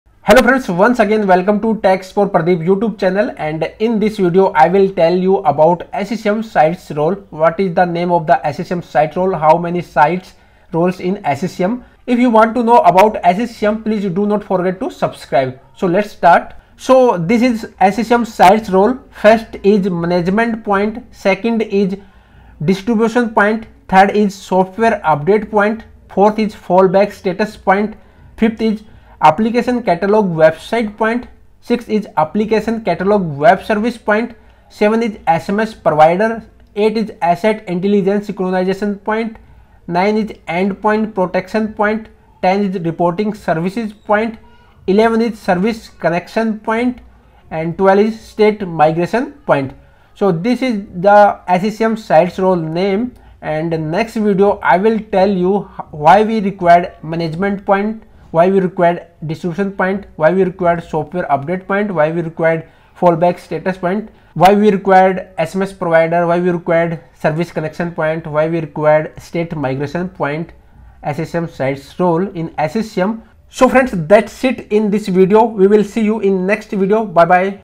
What is SCCM Site Roles | SCCM Site Role Name | SCCM SITE ROLES Details | Tech support pradeep
SCCM Site Role Name .

1-Management Point
2- Distribution Point
3- Software Update Point
4- Fallback Status Point
5-Application Catalogue Website Point
6-Application Catalogue Webservice Point
7-SMS Provider
8-Asset Intelligence Synchronization Point
9-Endpoint Protection Point
10-Reporting Services Point
11-Service Connection Point
12-State Migration Point

My Green Screen
Mic for Smartphone, PC, DSLR

हमें फॉलो करना न भूले Tech Support Pradeep --